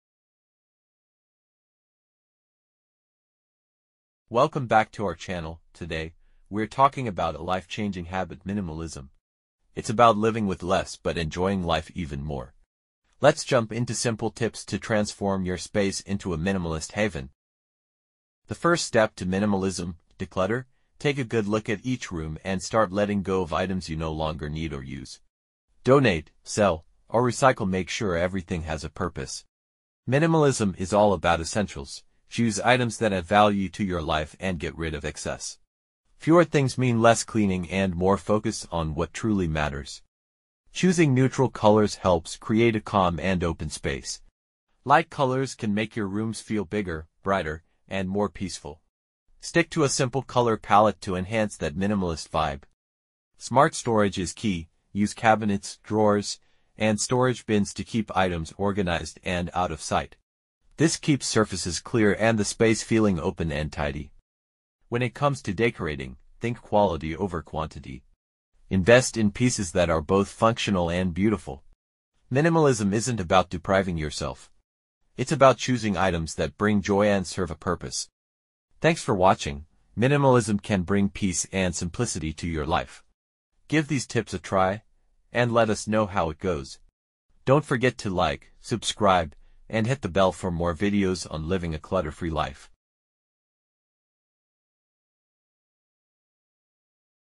Embrace Minimalism at Home 🏡✨ | Transform Your Space with Simple Steps!
Description:
Ready to declutter and find peace in simplicity? 🌱 Minimalism isn’t just a trend—it’s a lifestyle that can bring clarity and calm to your home. In this video, we’ll guide you through practical steps to embrace minimalism, from organizing essentials to creating mindful spaces that spark joy. Start small, think big, and watch your home (and mindset) transform! 🌿✨